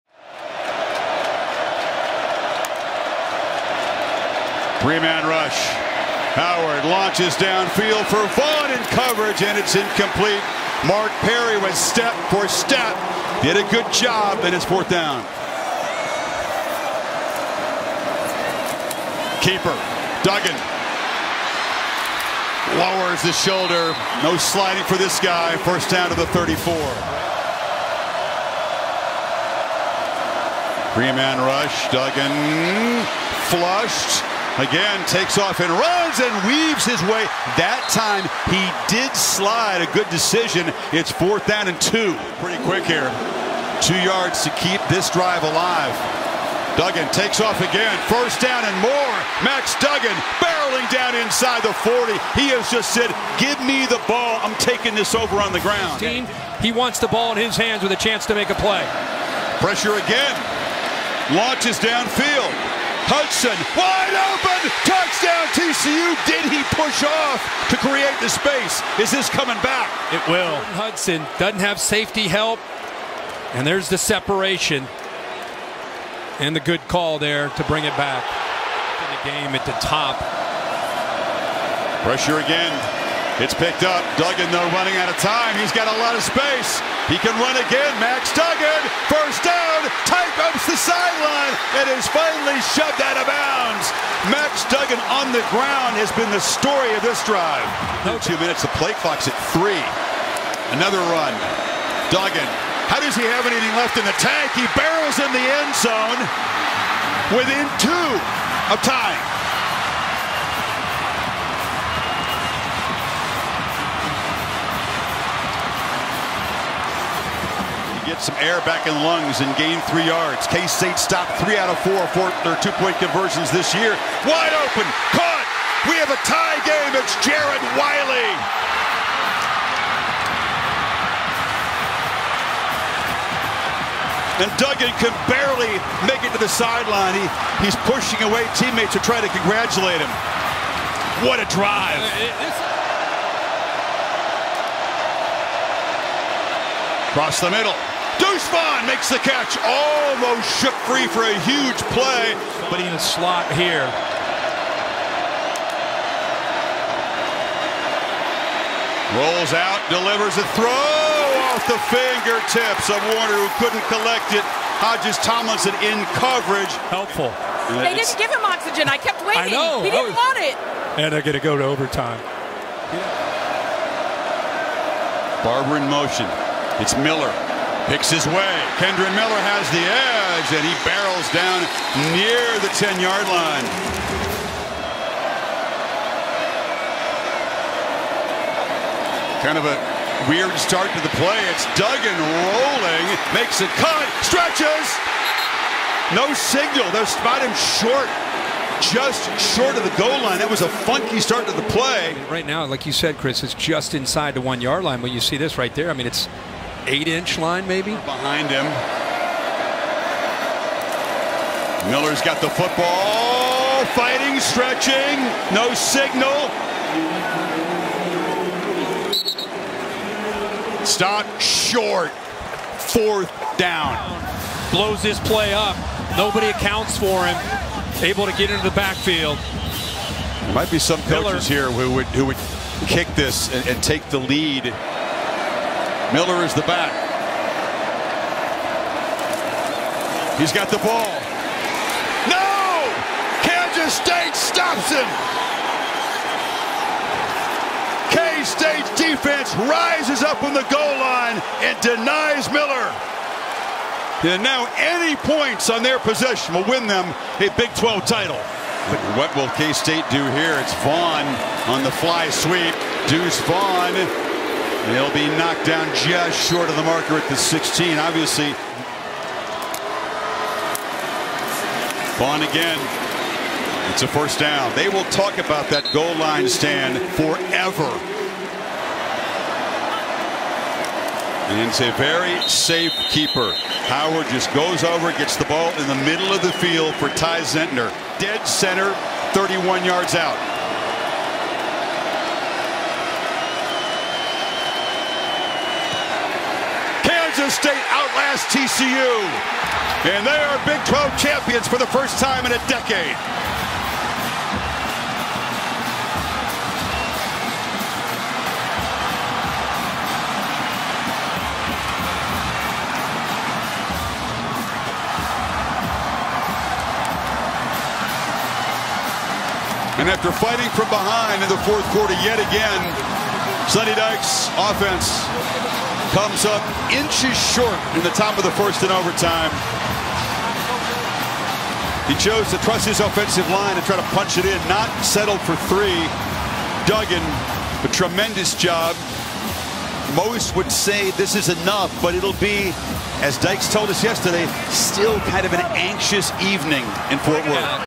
#10 Kansas State vs #3 TCU Exciting Ending | 2022 College Football
The college football game between TCU and Kansas State had an exciting ending as the two teams battled for a Big 12 Championship.